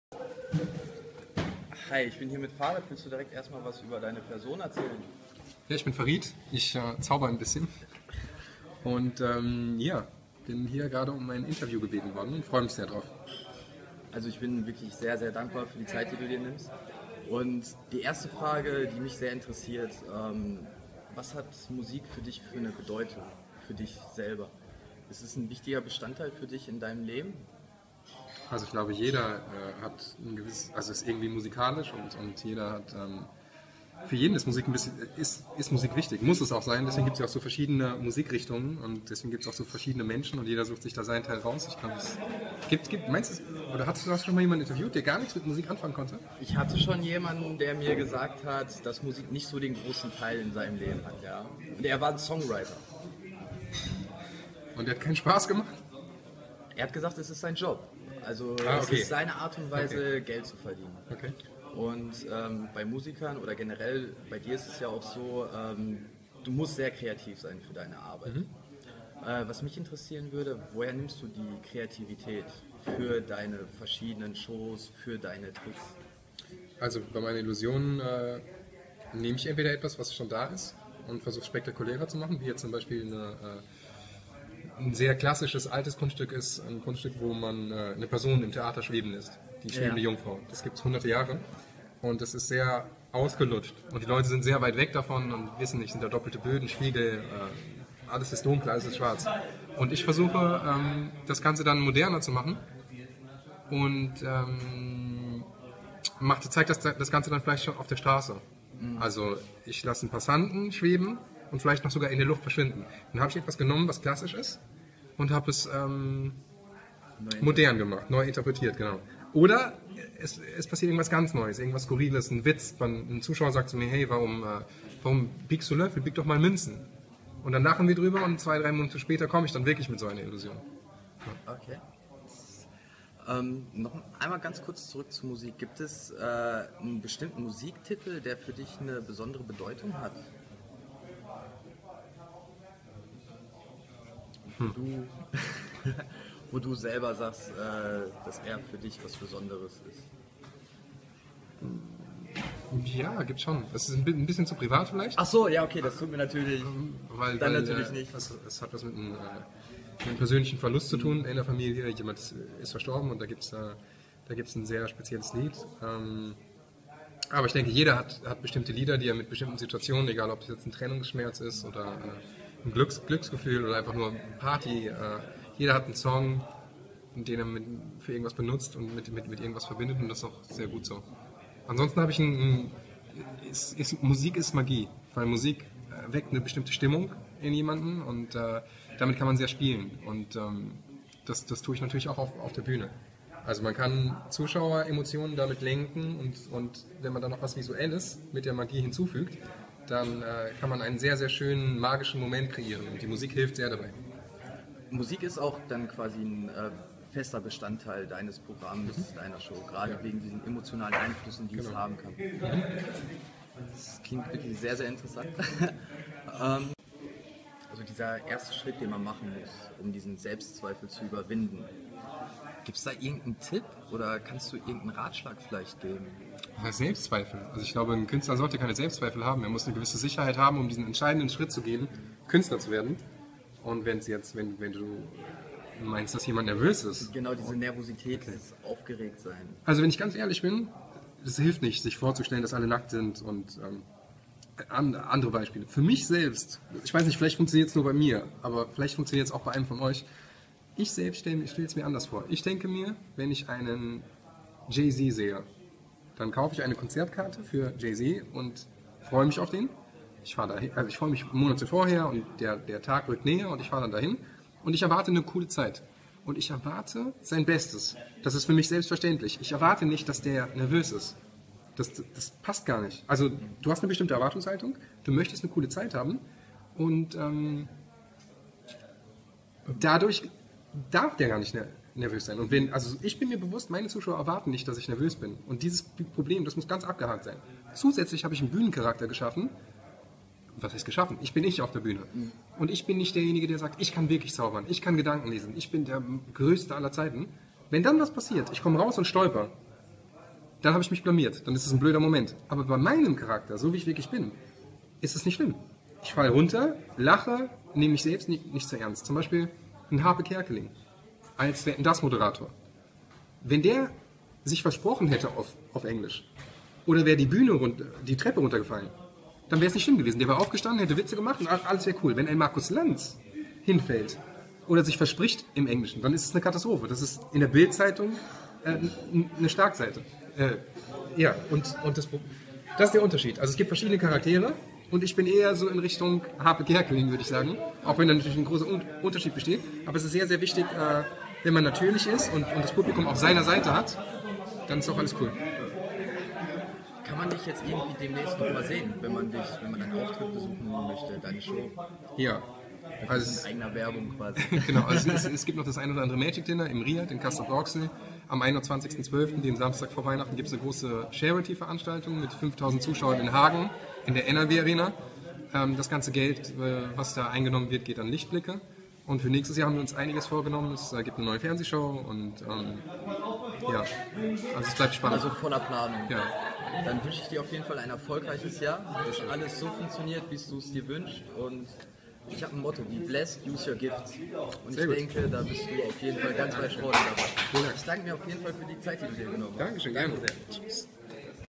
BlackINClement WOM Interview mit FariD dem Magier
BlackINClement WOM Interview mit FariD dem Magier/ Ende 2013 liFe aus dem Riad in NRW Germany. Vielen Dank nochmal an Mr. Air JamZ für seine spontane Interview Anfrage und selbstverständlich für die korrekte Art und Bereitschaft von FariD!

Farid hat persische Wurzeln und ist der erste deutsche Magier, der in 2007 von Sony BMG unter Vertrag genommen wurde.

Mit verschiedenen Live Shows, wie „Inside Magic -- Werde Teil der Show" oder seiner "Magic Dinner Show" tourt er durch Deutschland und verzaubert die Menschen!

Im Interview erfahrt ihr etwas mehr zu seiner Person und seinen Ansichten und lernt eine sehr symphatische Seite von ihm kennen!